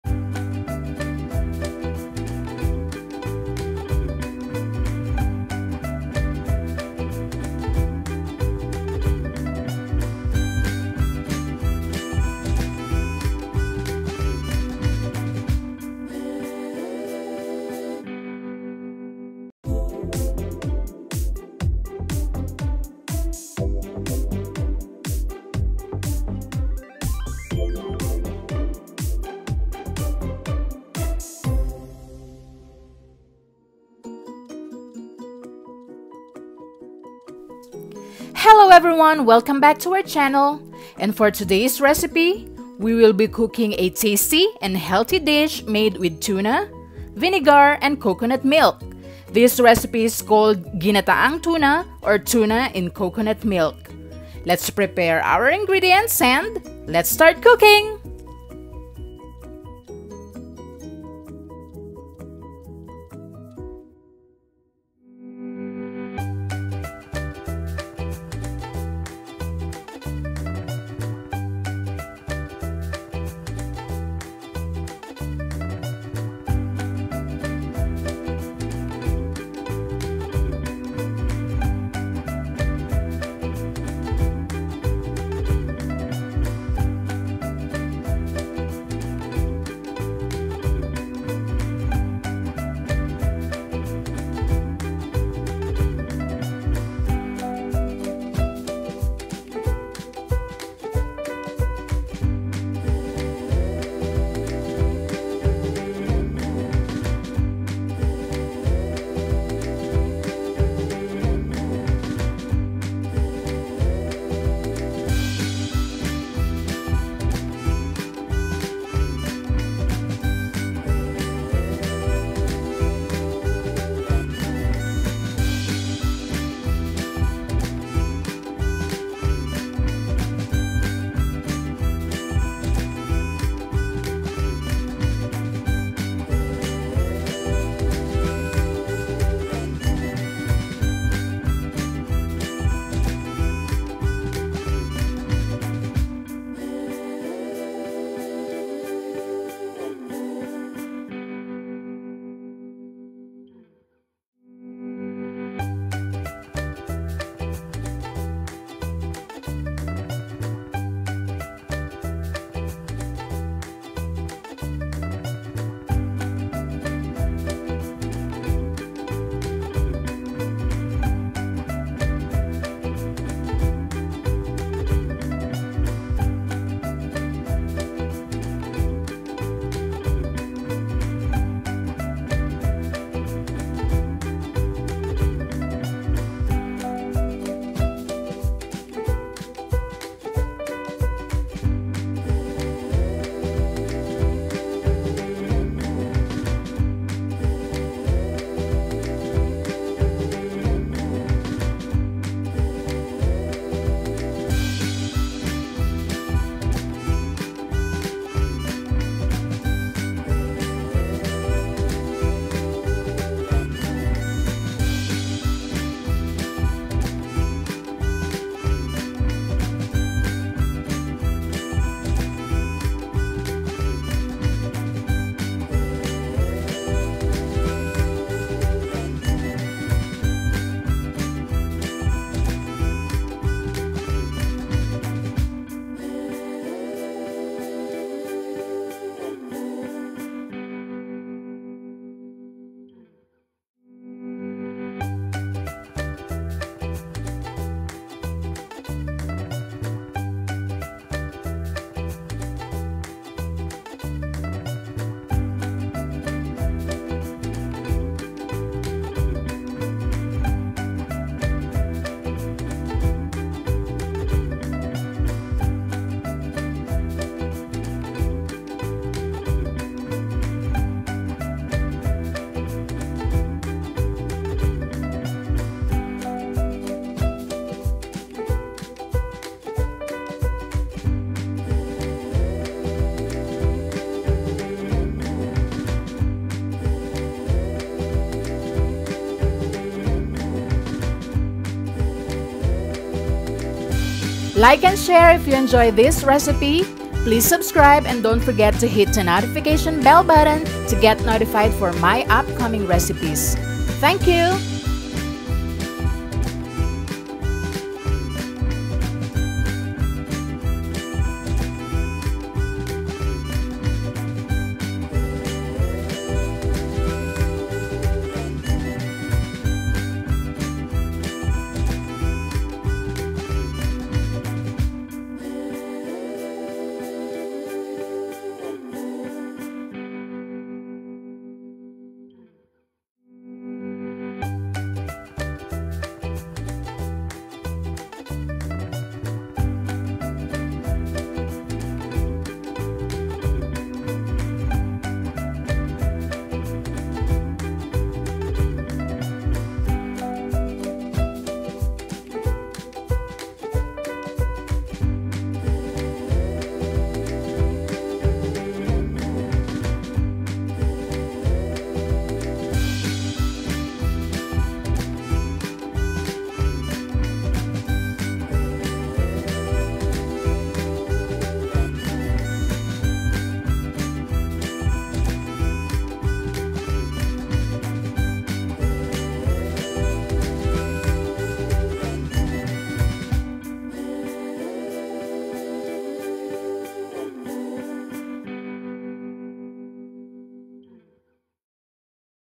GINATAANG TUNA RECIPE | HOW TO COOK FISH IN COCONUT MILK | Pepperhona’s Kitchen
Hello Everyone,
In this video, I will be showing you my way of cooking tuna fish in coconut milk. This recipe is very tasty, healthy and easy to cook. 😊

Happy Cooking 🧑🏻‍🍳

Love,

Pepperhona ♥️